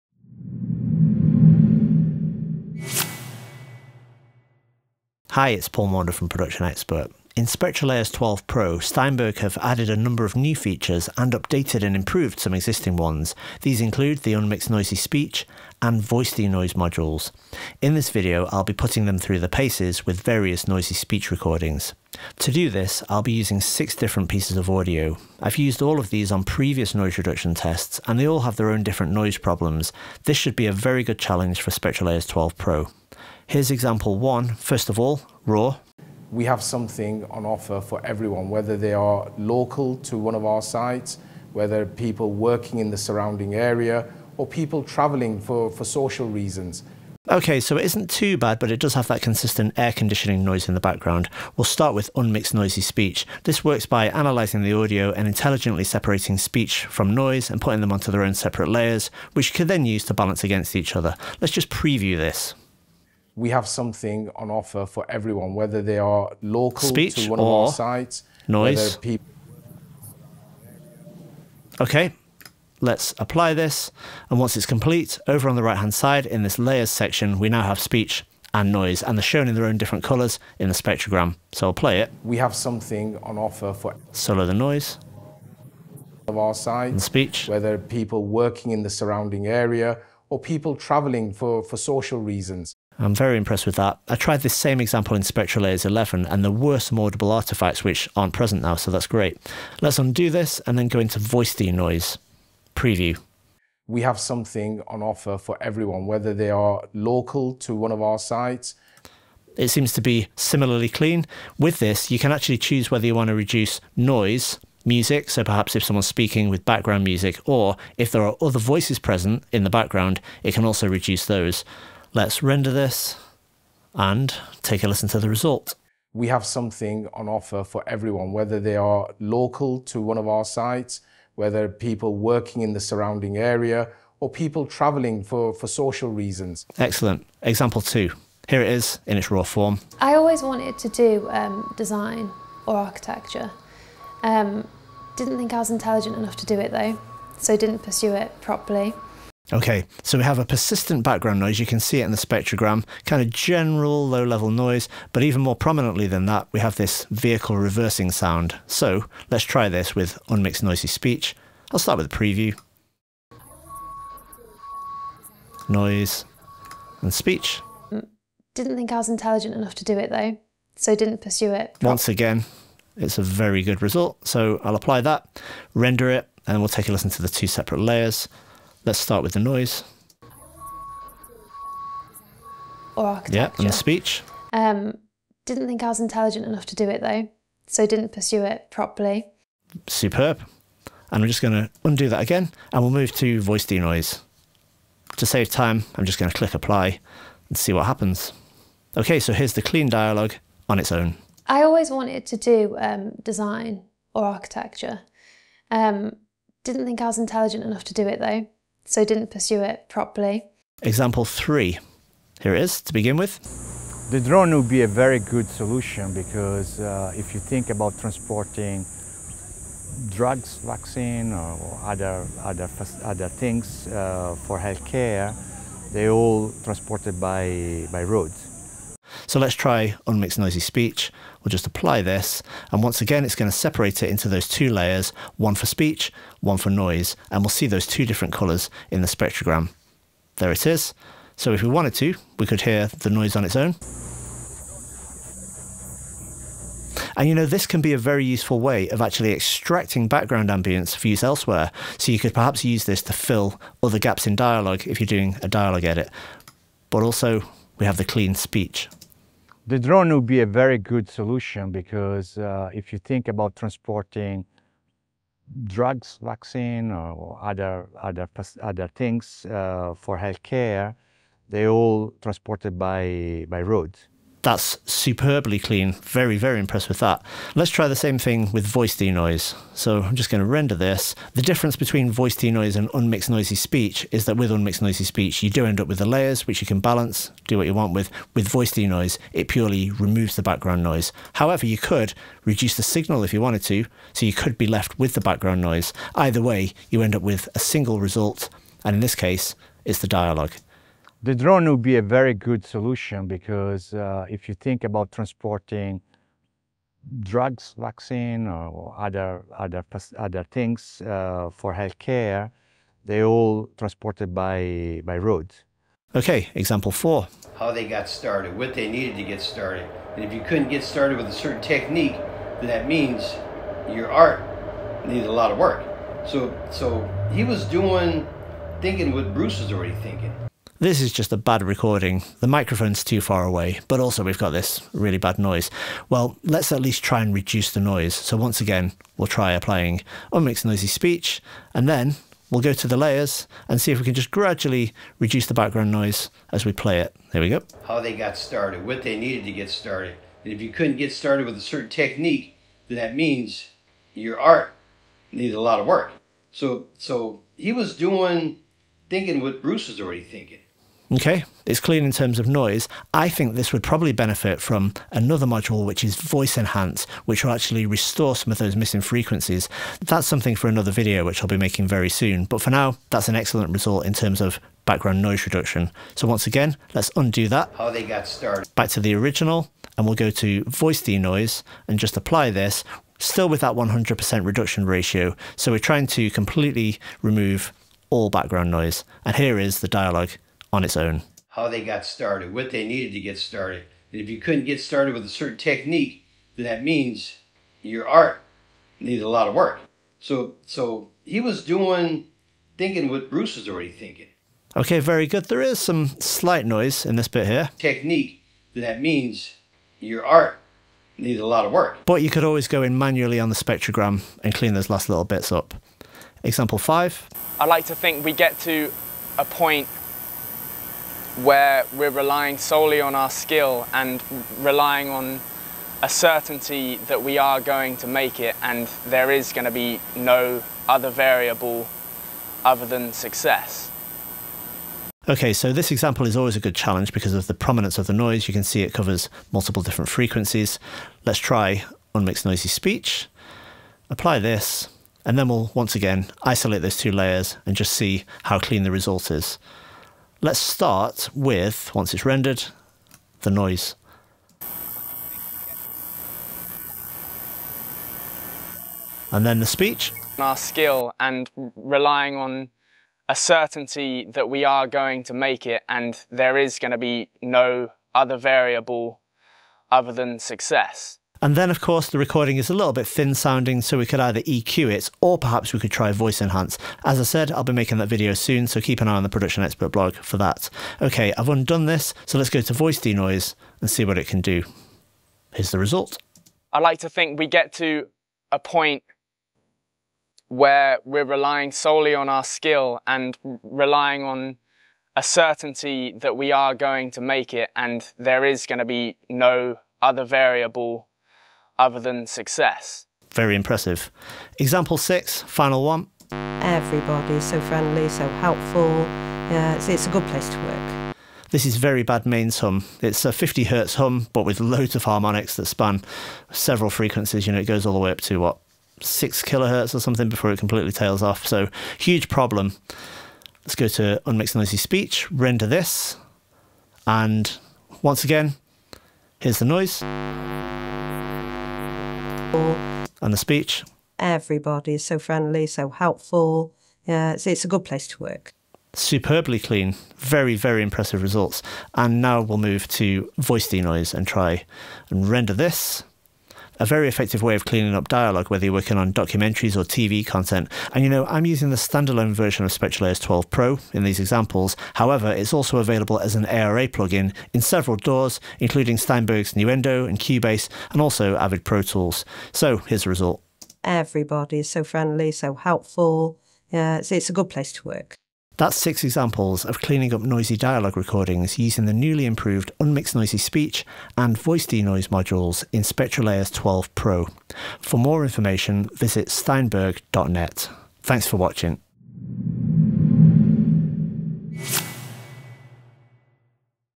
Testing Steinberg SpectraLayers 12 Pro's Unmix Noisy Speech and Voice DeNoise
As well as brand new features in new SpectraLayers 12, Steinberg have improved several existing modules in this release. Here we focus on two which are dialogue focused; Unmix Noisy Speech and Voice DeNoise, putting them through their paces on a number of challenging, noisy dialogue recordings.